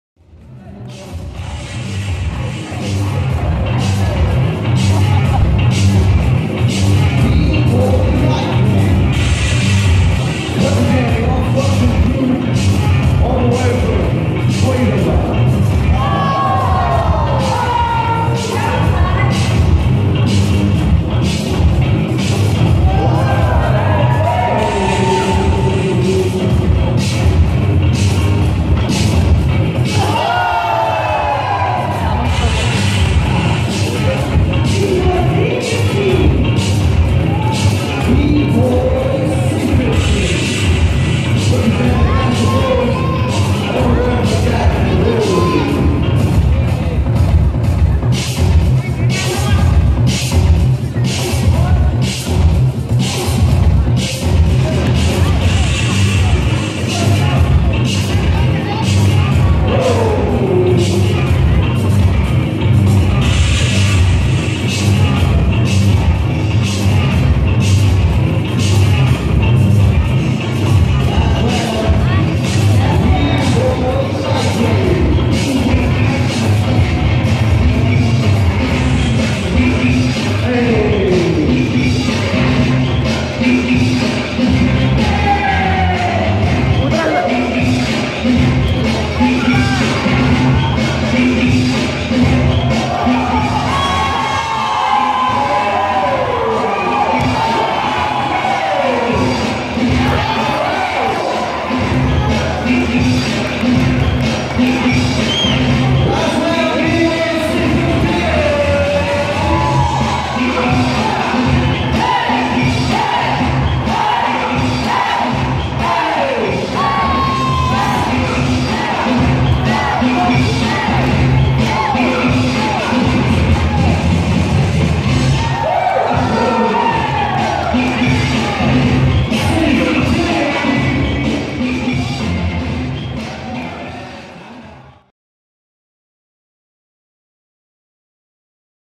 LiteRay vs C15 (Top 8) // BreakStation
Challenge Cup Nepal 2016
Bboy LiteRay - Winner